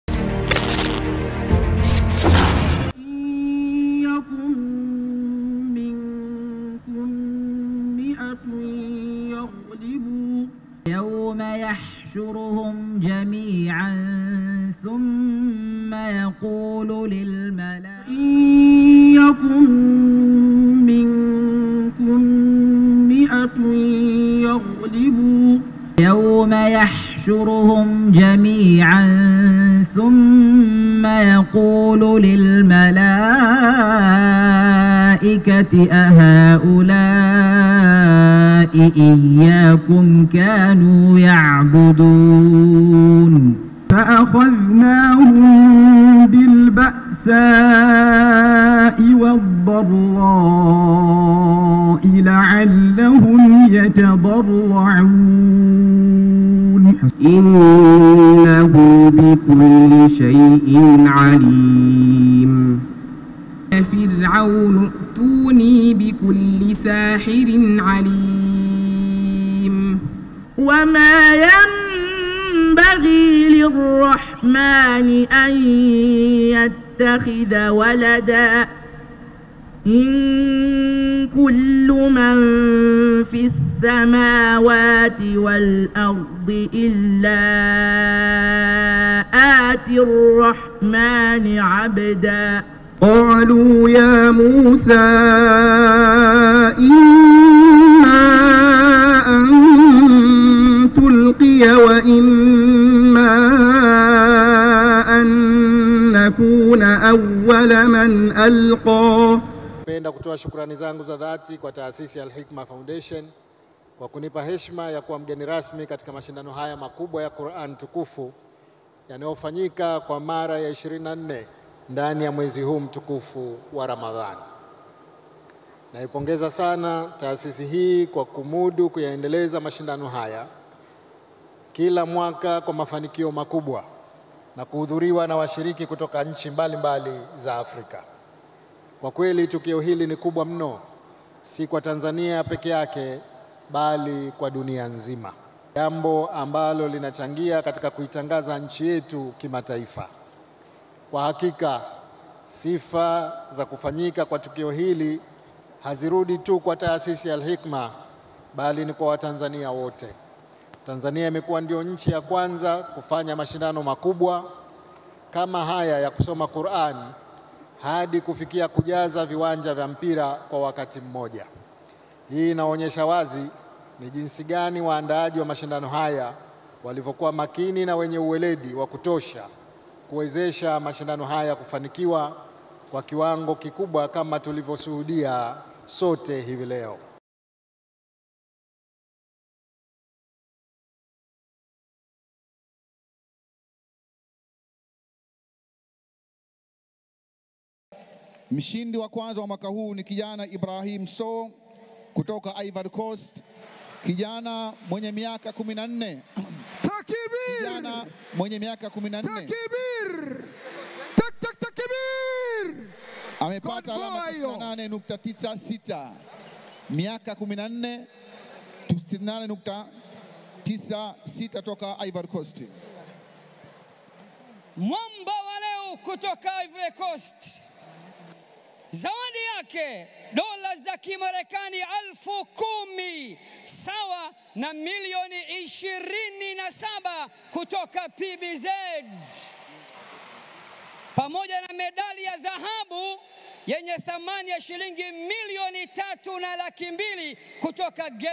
DAKIKA 04 ZA RAIS MWINYI FINALI ZA 24 YA MASHINDANO YA QURAN TUKUFU AFRIKA/MSHINDI HUYU HAPA.
Rais wa Zanzibar na Mwenyekiti wa Baraza la Mapinduzi Dk.Hussein Ali Mwinyi akizungumza katika fainali za 24 za Mashindano  ya Quran  Tukufu  Afrika yaliyoandaliwa na Taasisi ya Al Hikma Foundation kwa kushirikisha  nchi mbalimbali 22 za Afrika na nje ya Afrika katika Uwanja wa Benjamin Mkapa, Dar es Salaam  tarehe 24 Machi 2024.